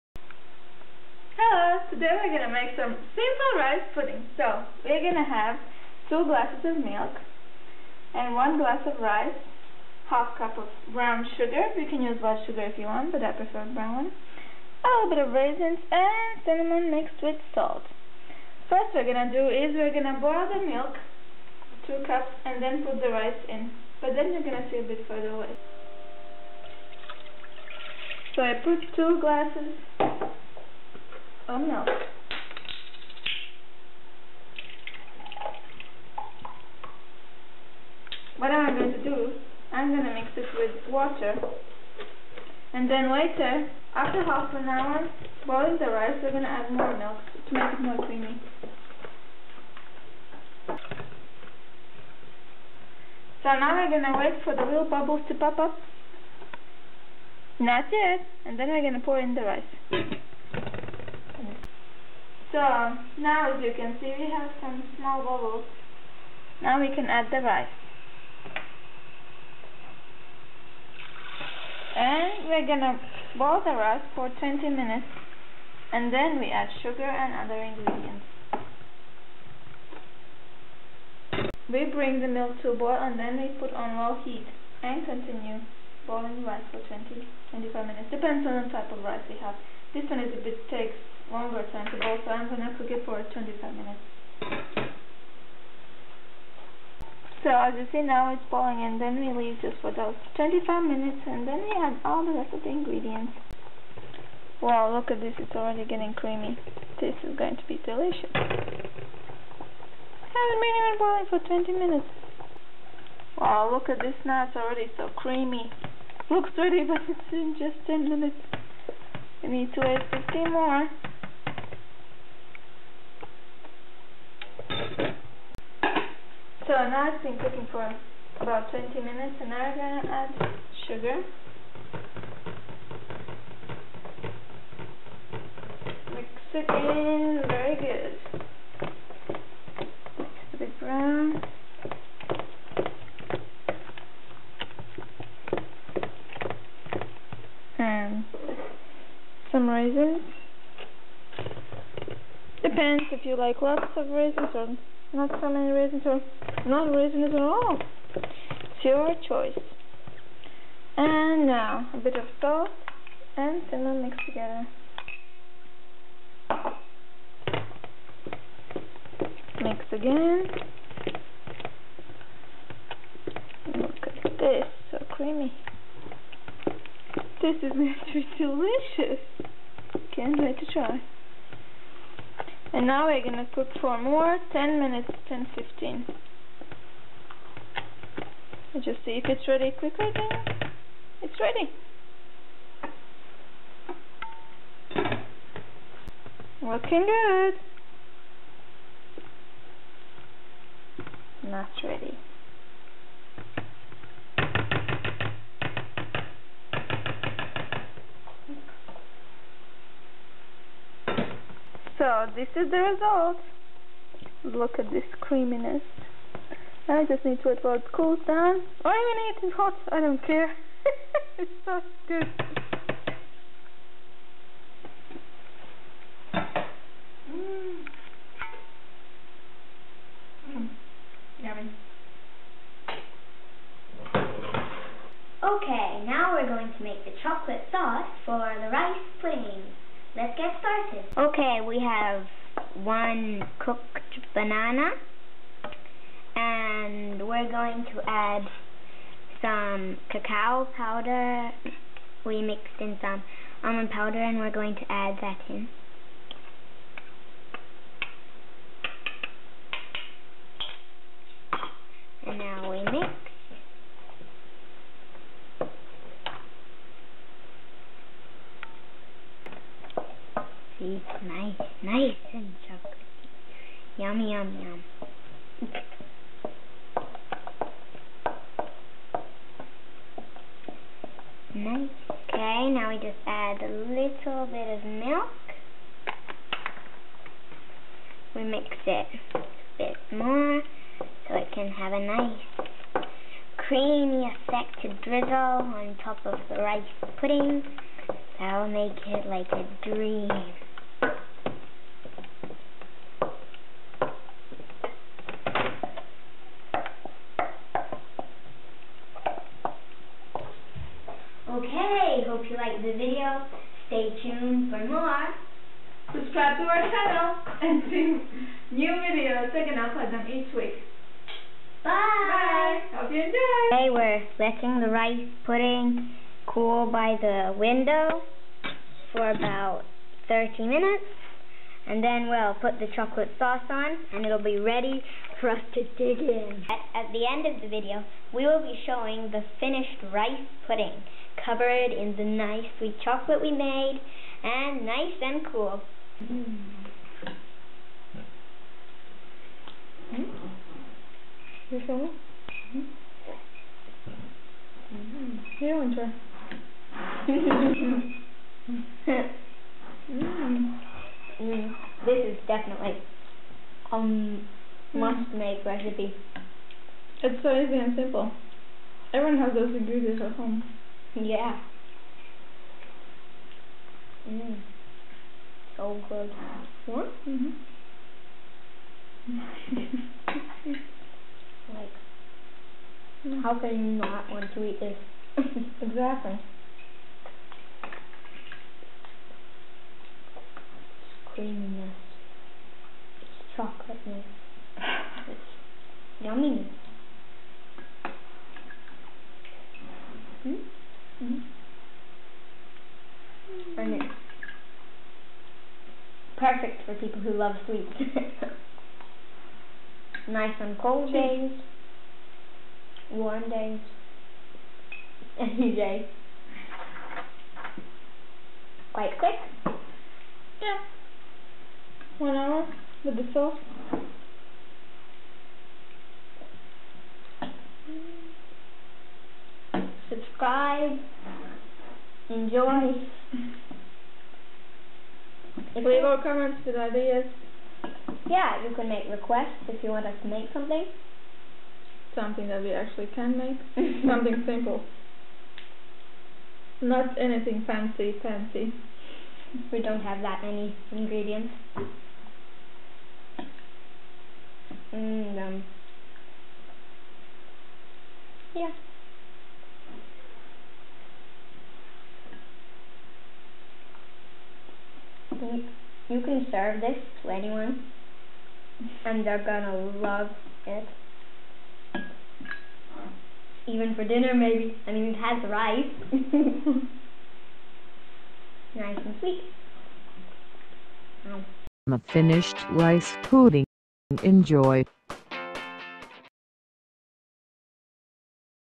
Friends in the Kitchen - Rice Pudding
Friends in the Kitchen,  On the menu for today is: Rice Pudding (with Chocolate Sauce.)